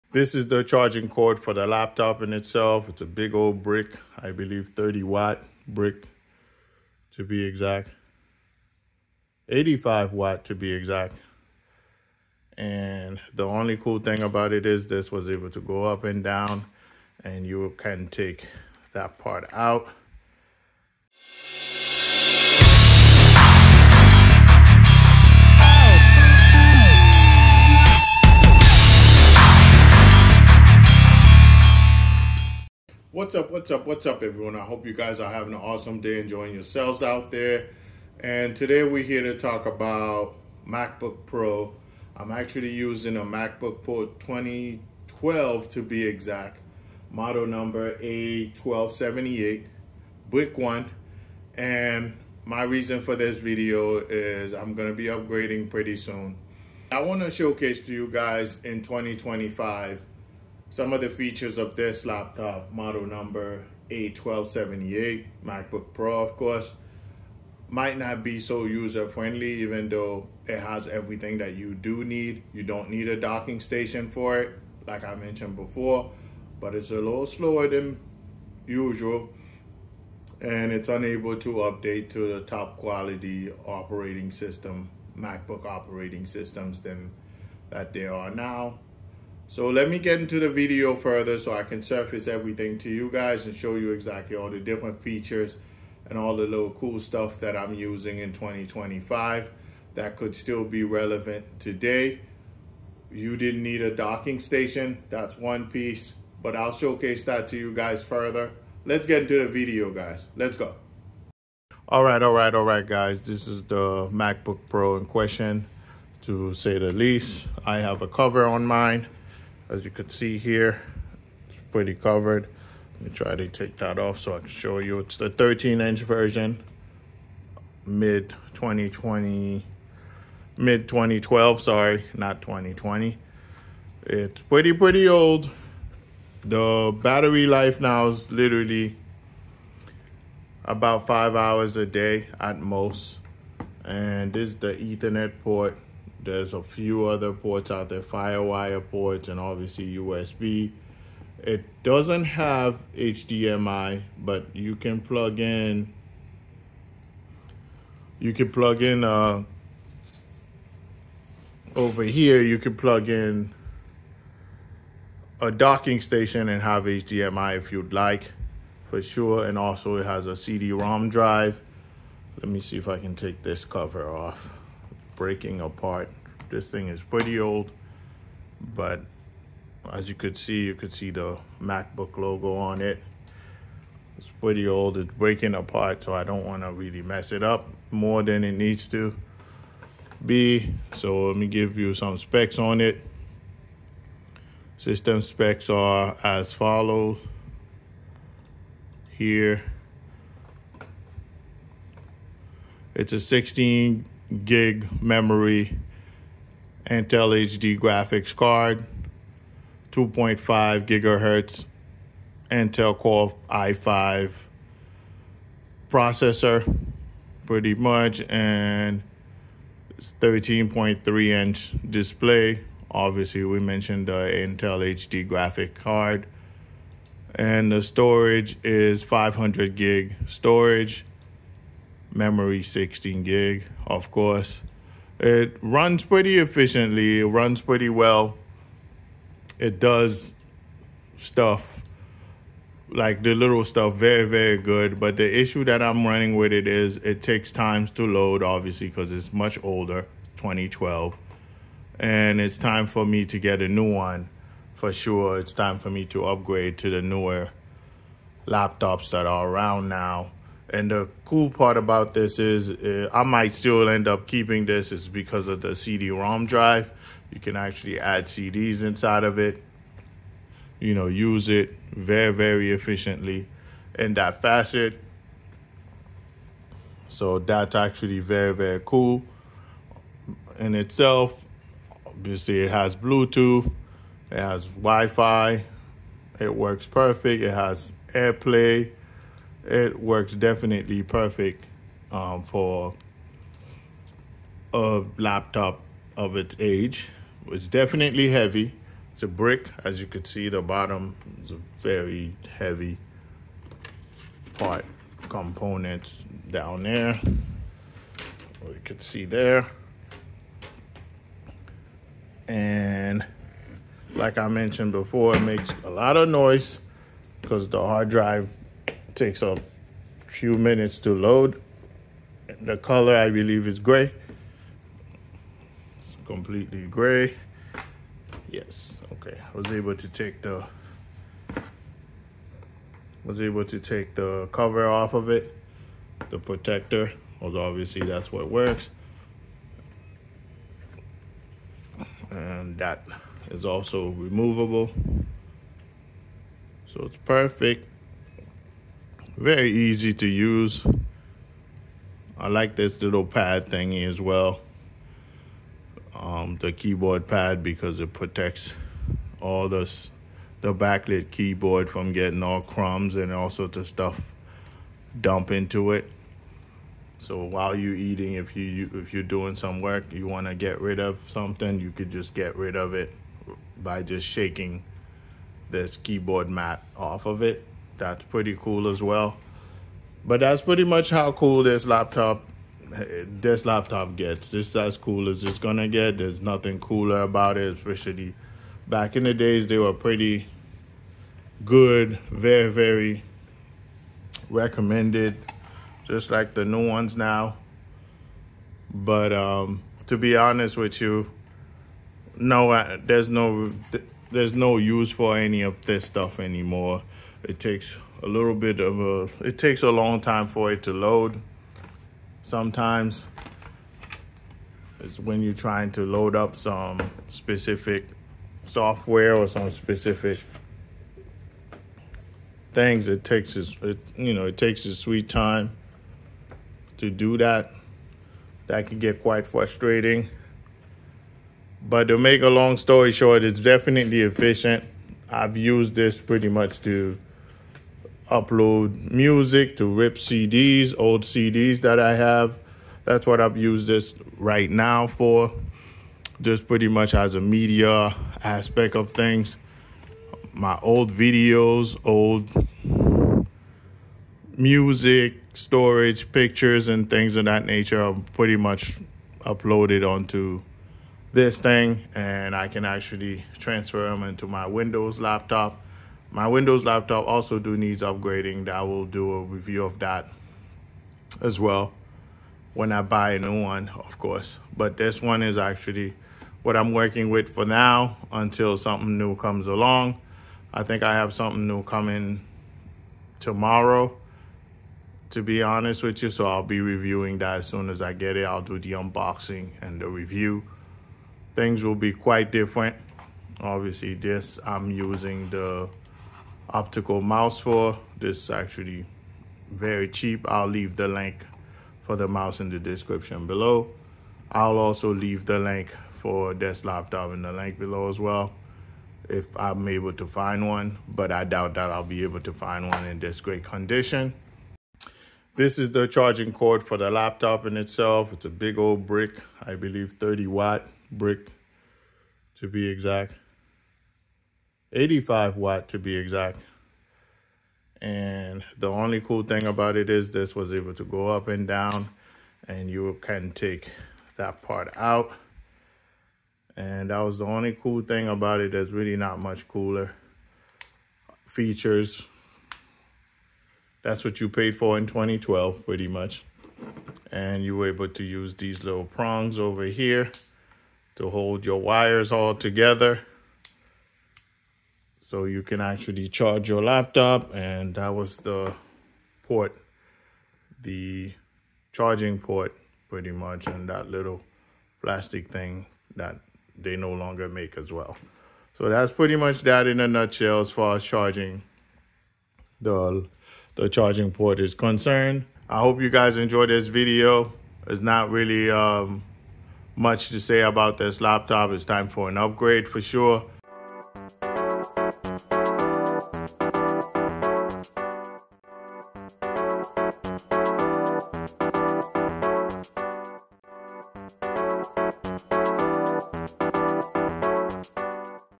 Using A 2012 Macbook Pro in 2025
Features:

Silver color
MacBook Pro
Intel Core i5-3210M Dual-Core 2.5GHz processor
Max Turbo Frequency: 3.1 GHz
Intel Smart Cache: 3 MB
16 GB DDR3 SDRAM
500 GB SATA hard drive
Slot-loading SuperDrive DVD±RW DL drive
Intel HD Graphics 4000 graphics
Integrated audio with built-in stereo speakers
Built-in omnidirectional microphone
Built-in 10/100/1000 Gigabit Ethernet
Built-in 450 Mbps AirPort Extreme wireless LAN w/MIMO (802.11n)
Built-in Bluetooth 4.0
Built-in 720p FaceTime HD camera
Full-size, illuminated keyboard with ambient light sensor; scrolling Trackpad
13.3-inch TFT widescreen LED-backlit LCD display (1280 x 800 resolution)
SDXC card slot
Kensington cable lock slot

I/O Ports:
Thunderbolt port
Two (2) USB 3.0 ports
FireWire 800
RJ-45 Ethernet port
Combined optical digital audio input/audio line-in mini-jack
Combined optical digital audio output/headphone out mini-jack
SDXC card slot

Battery Specifications:
Built-in lithium-polymer battery
63.5-watt
5 hours

Power Adapter Specifications:
16.5V, 3.65A

Unit Dimensions:
0.95 x 12.78 x 8.94-inches (H x W x D)

Package Includes:
Apple MacBook Pro 13.3" Notebook
MacBook Pro Power adapter